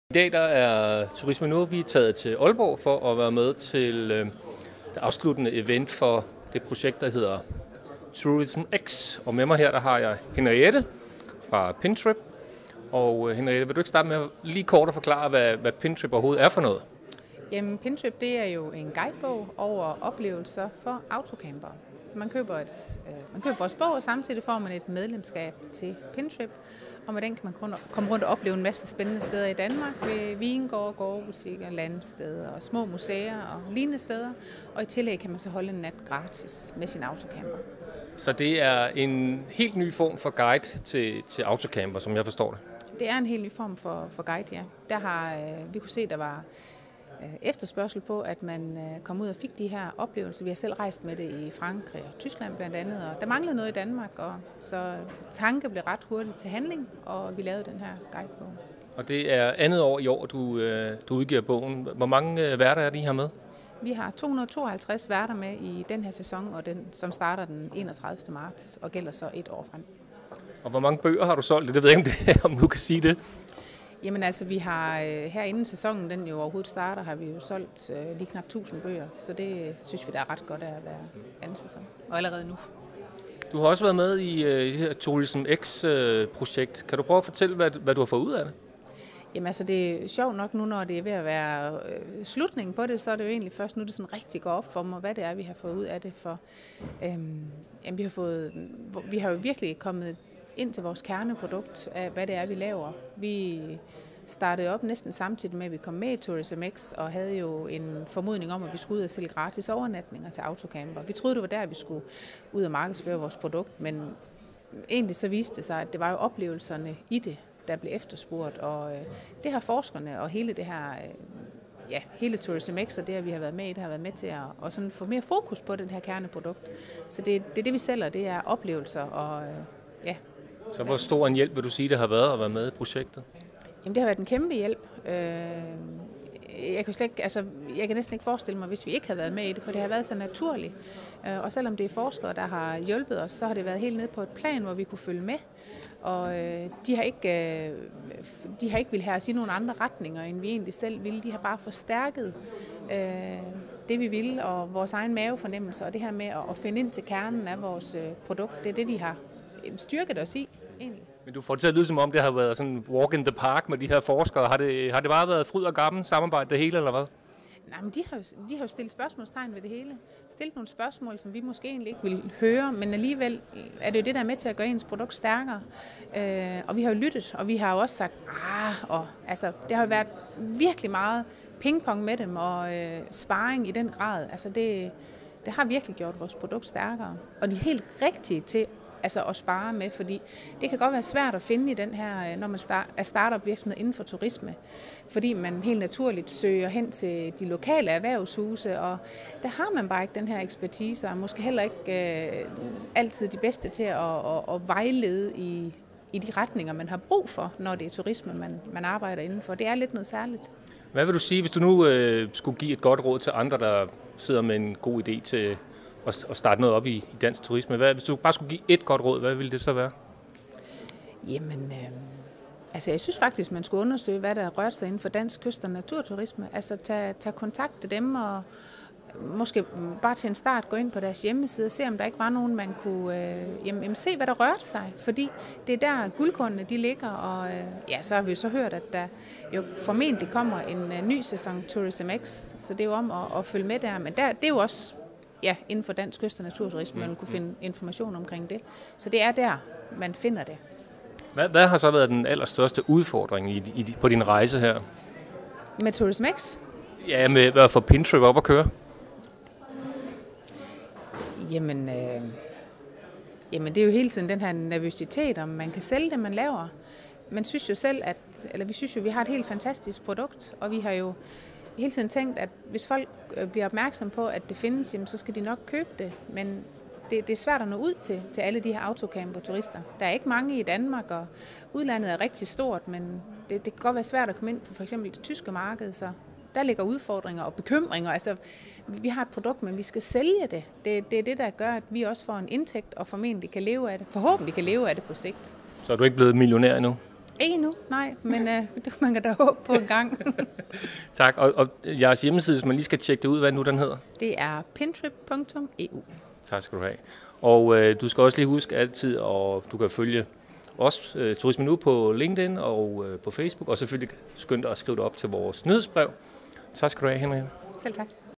PintripTourismXTurismeNu
Henriette Rask fra Pintrip fortæller, hvordan det er at være iværksætter i dansk turisme. Turisme.nu mødte Henriette ved det afsluttende event for acceleratorforløbet TourismX.